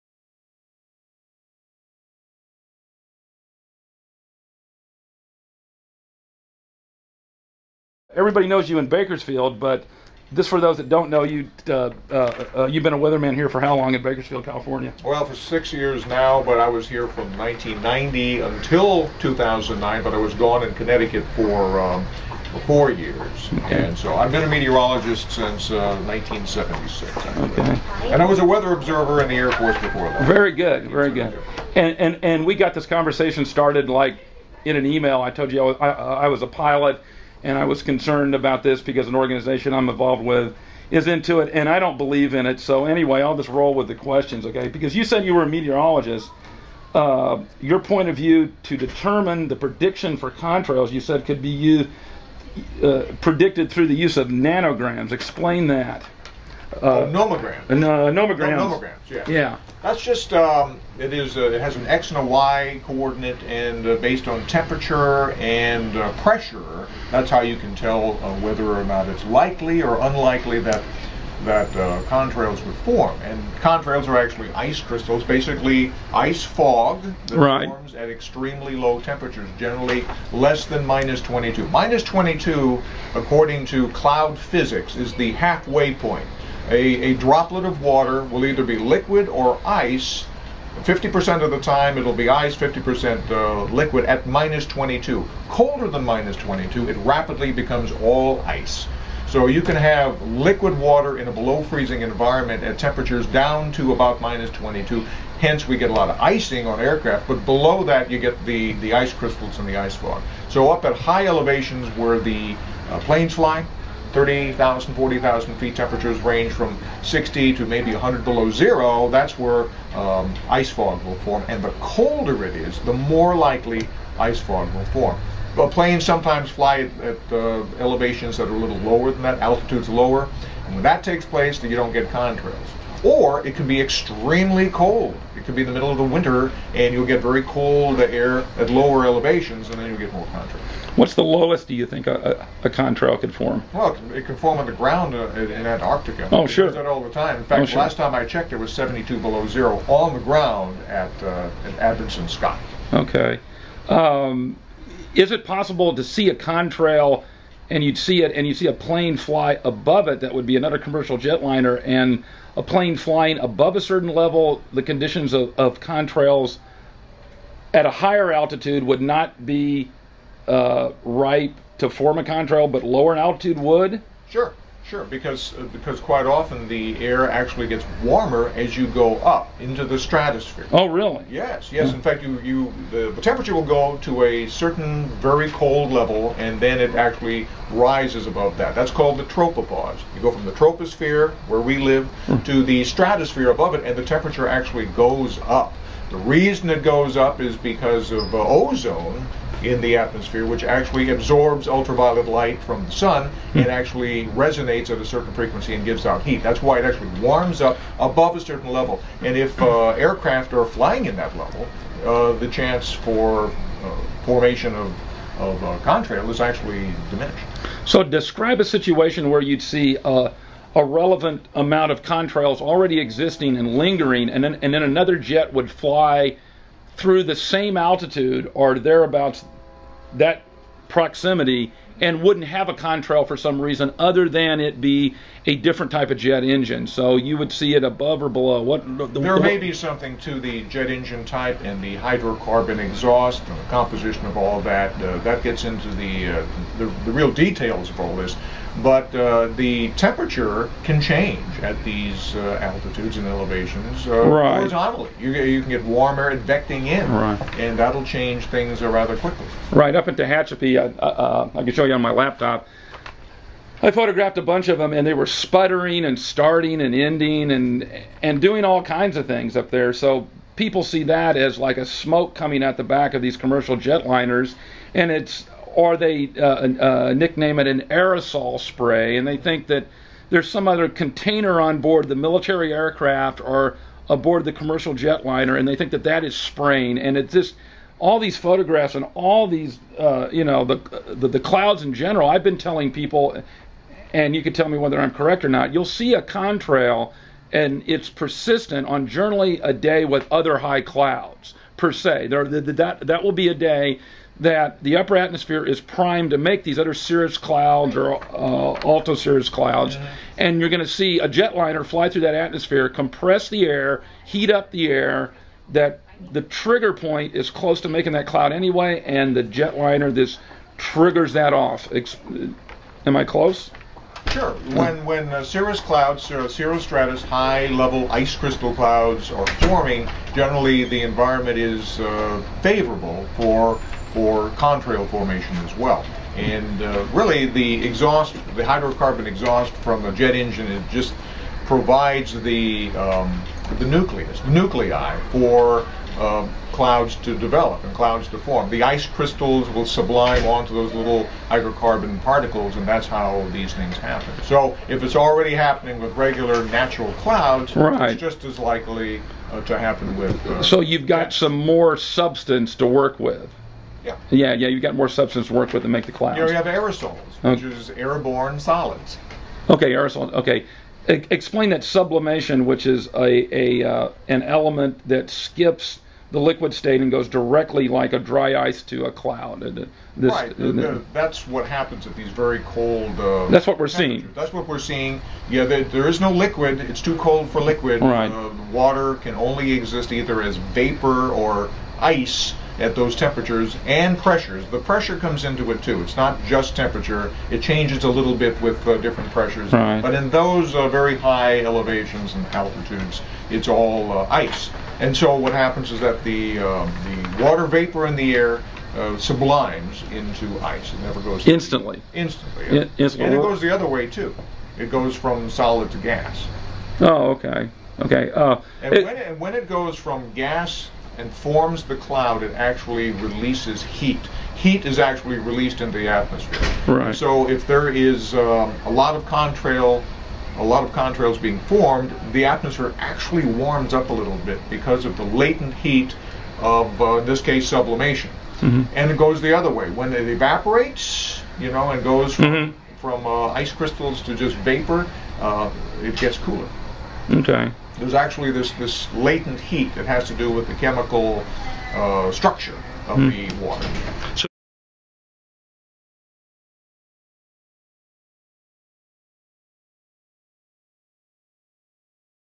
Chemtrails Part I ? Chief Meteorologist Miles Muzio
Miles Muzio
Chief Meteorologist
KBAK-TV
 CBS 29 News
Bakersfield  California

This is an ongoing controversy with some people who insist a malevolent conspiracy is subliminally gassing Americans.

I have found no credible evidence of this. Condensation trails are common and form as a function of temperature. Condensation nuclei are produced by hydrocarbon exhaust and quickly form ice fog in the high atmosphere. The colder it is at flight level the more persistent the ice fog is.

There are really three types of fog (which are manifest as clouds aloft): 1) regular water droplet fog at temperatures above freezing, 2) water droplet fog at temperatures below freezing (which can result in icing conditions for aircraft), and 3) ice crystal fog (ice fog). Ice fog generally forms at temperatures below -22F. In arctic towns during the dead of winter it is not unusual to have ice fog form behind cars on the road. Ice fog dissipates very slowly because it must sublime from solid to vapor which requires a molecular heat transfer of about 540 calories per gram. At extremely low temperatures in the 30,000 to 40,000 foot range (between -60 and -100 degrees F), the change of state can take some time.

When I was a military meteorologist in Alaska back in the 1970s, one of the routine forecasts I put out was a contrails prediction. It was something that dated back to World War II. Our bombers needed to know at what altitude they must fly in order to NOT produce contrails. Of course, there was radar back then and the Japanese probably could spot an incoming B-29, but perhaps not. If contrails were visible 100 miles away then everyone would know. Nomograms were developed for the prediction. It was fairly straight forward. A critical temperature and pressure was required for contrail formation, usually something like -45F. Humidity came into the equation for relatively warm temperatures, but once it got below about -70F humidity didnt matter- ice clouds would form regardless. When you see jets in which the contrail forms for only a short distance behind the aircraft and then it quickly disappears- that means the jet is flying at an altitude close to the critical contrail temperature.

I can also tell you that the manner in which contrails disperse, or dont disperse reveals important information about atmospheric stability and diffluence. The same is true about lenticular clouds that form in Tehachapi. These mountain wave clouds dont automatically form when its windy. There are several other considerations, such as stability, humidity and the wind angle relation to the mountain range. In the same way contrails will expand in areas of diffluent flow (winds pulling apart over a horizontal area). Much of this also applies to steam or exhaust that is released from a common smoke stack in breezy conditions. If the smoke goes up or if it goes down, if it expands or if it stays uniform- all tell a story about the character of the atmosphere.

So when contrails form an X in the sky, it means there are two persistent contrails behind aircraft with tracks at 90-degrees to each other- nothing more.